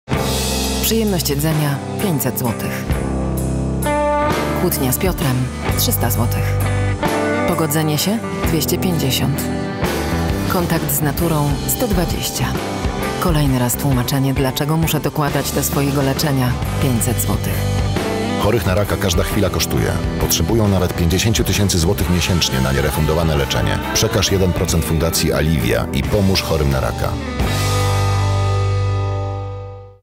CENNE CHWILE ŻYCIA
W Polsce dostęp do nowoczesnych terapii onkologicznych jest mocno ograniczony i nierówny w różnych województwach. Będąc polskim pacjentem, chory często staje przed wyborem – skorzystać z najnowocześniejszych, najbardziej skutecznych metod za własne pieniądze, lub leczyć się metodami refundowanymi, ale dającymi mniejsze szanse na opanowanie choroby. Nie jest to wybór łatwy, przede wszystkim dlatego, że leczenie onkologiczne jest bardzo drogie i nieraz wiąże się z wyjazdem zagranicę – do ośrodka, w którym dana metoda jest z powodzeniem stosowana. Często też pacjenci w poszukiwaniu szansy na życie dojeżdżają do odległych ośrodków leczenia w kraju.

Dlatego fundacja Alivia pomaga chorym na raka w finansowaniu nierefundowanych terapii, prywatnych konsultacji, niezbędnych badań i dojazdu do ośrodka leczenia. Ułatwia również powrót do sprawności i aktywnego życia, refundując wydatki na niezbędne artykuły medyczne i rehabilitacyjne.

Fundacja Alivia w ramach programu pomocy przekazała
pacjentom onkologicznym już ponad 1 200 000 zł
na ratowanie ich zdrowia i życia.

Przekaż swój 1% podatku Alivii i pomóż chorym na raka.

Kampania powstała dzięki zaangażowaniu PRO BONO następujących osób i firm:

Koncept i nadzór nad realizacją:
Agencja Young & Rubicam
Creative Director - Radek Kotapka
Art. Director - Kasia Cergowska
Copywriters: Olga Górniak & Tomasz Zieliński "Owca"
Producer - Piotr Owsianka
Account Manager - Romek Lubczyński

Realizacja spotu:
Opus Film – Sonia Pośpiech, Marcin Pośpiech, Renata Łysik
Reżyser – Tomek Nalewajek
DOP – Bartek Bieniek
Scenograf – Ola Kierzkowska
Kostiumy – Weronika Wyrzykowska
Ekipa filmowa
Post Produkcja – Rio de post
Studio dźwiękowe – Ucho
Muzyka – Konsultacje Muzyczne

Aktorzy:
Katarzyna Warnke
Piotr Stramowski

Lektorzy:
Justyna Gardzińska
Jacek Król

Wszystkim serdecznie dziękujemy!